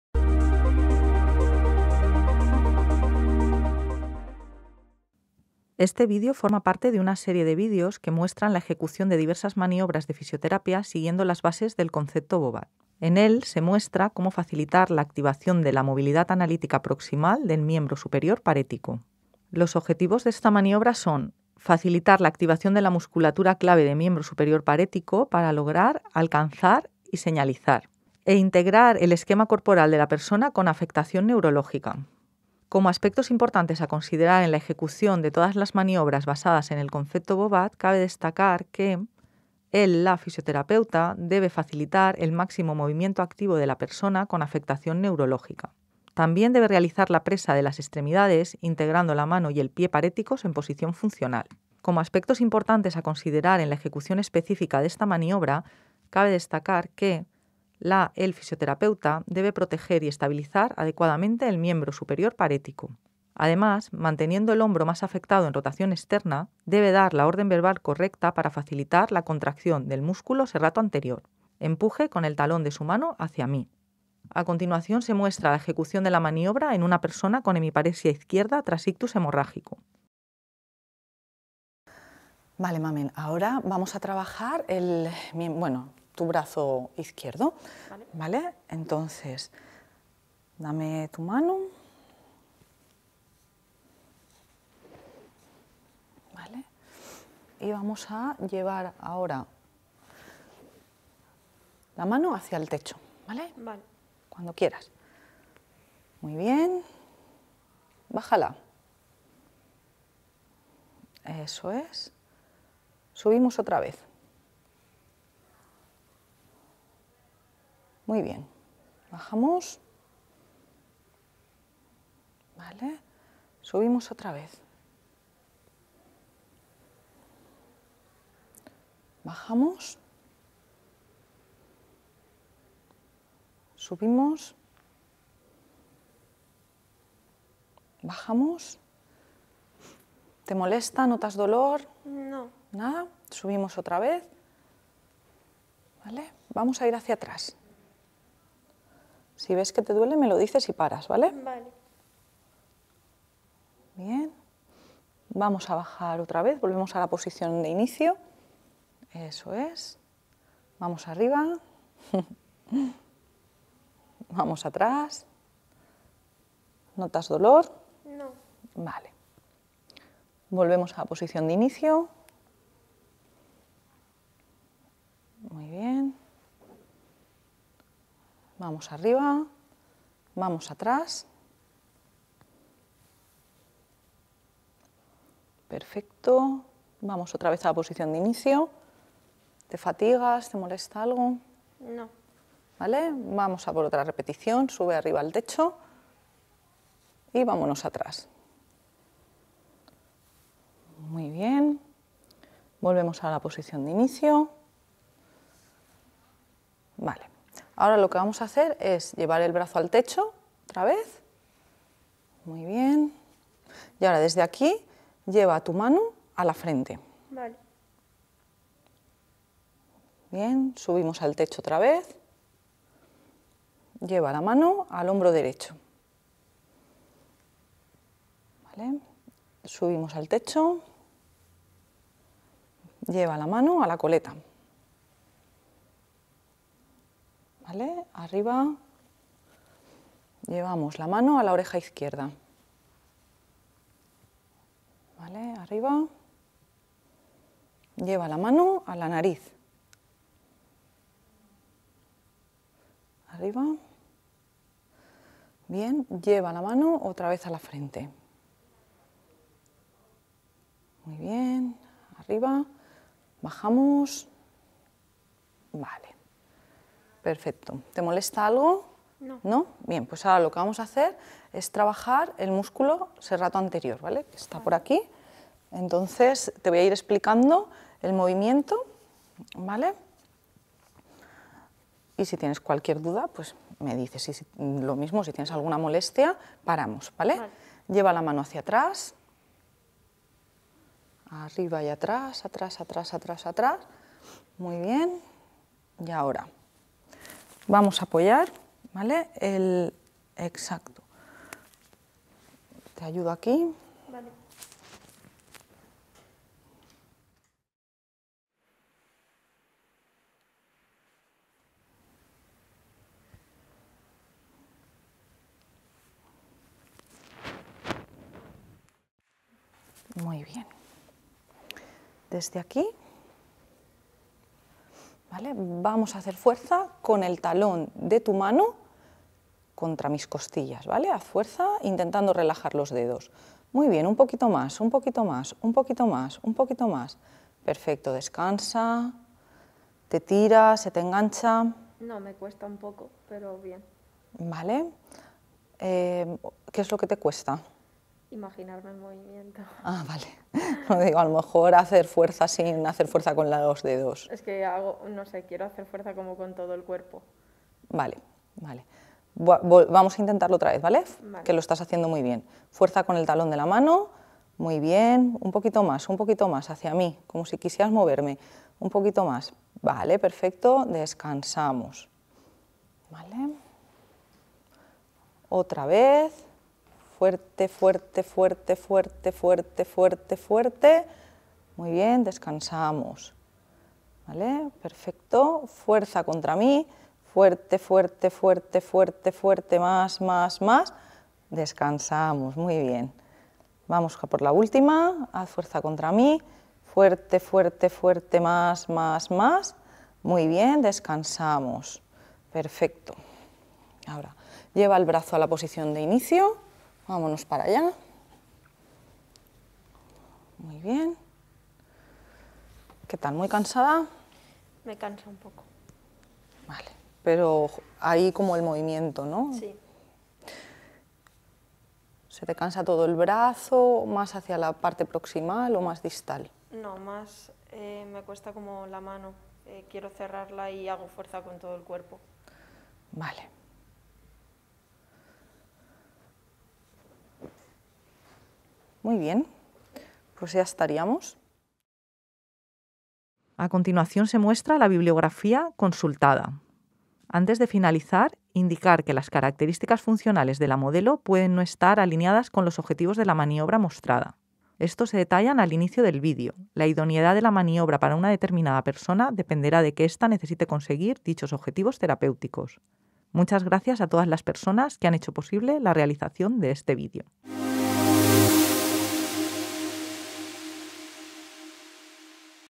Activación de la movilidad analítica proximal del miembro superior parético
Autora principal: Mª Luz Sánchez Sánchez
Autores sencundaris: Natalia Cezón Serrano, María Arántzazu Ruescas Nicolau, Constanza Isabel San Martín Valenzuela, Cristina Flor Rufino, Anna Arnal Gómez, María del Carmen Sobrino Sánchez y
Rafael García Ros.

Este vídeo forma parte de una serie de vídeos que muestran la ejecución de diversas maniobras de fisioterapia siguiendo las bases del Concepto Bobath.
En él se muestra cómo facilitar la activación de la movilidad analítica proximal del miembro superior parético, en una mujer con hemiparesia izquierda tras ictus hemorrágico.
Cabe destacar que las características funcionales de la modelo pueden no estar alineadas con los objetivos de la maniobra mostrada. Estos se detallan al inicio del vídeo. La idoneidad de la maniobra para una determinada persona dependerá de que esta necesite conseguir dichos objetivos terapéuticos.